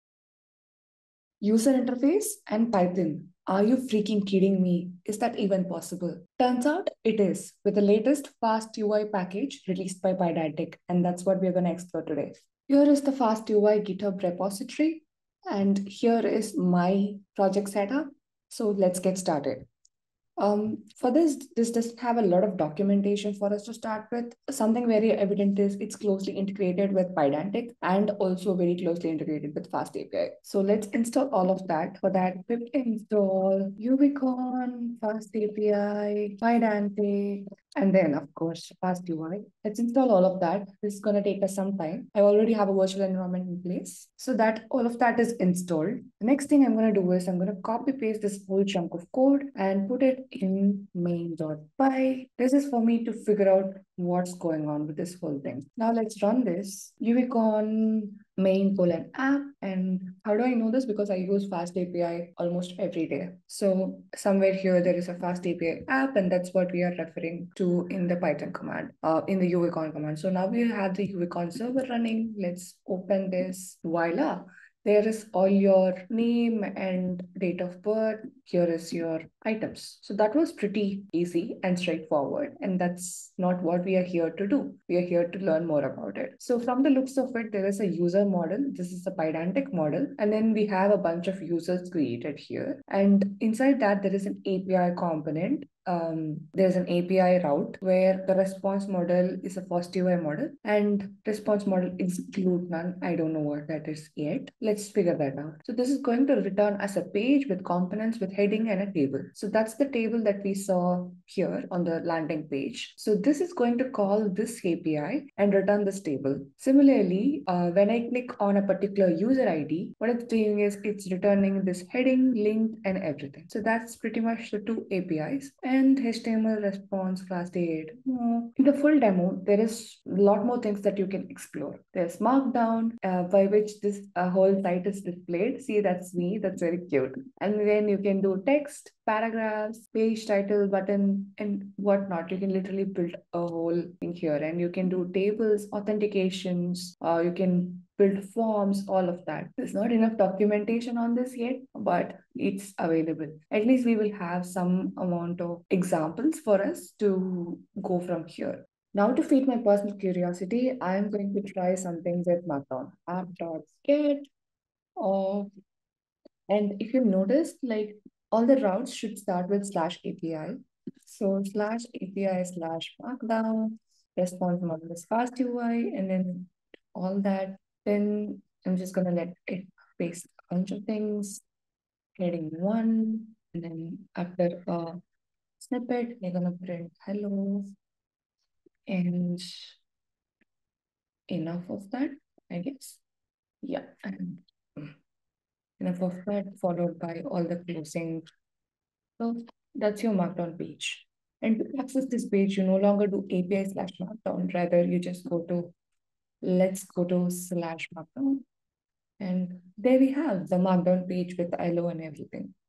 Building UI with Python's FastUI Library
UI in Python is a long-term dream of all Pythonistas. The FastUI library is on a mission to make it a reality.

In this video, we will understand the basic example given by fastui and extend it to meet our requirement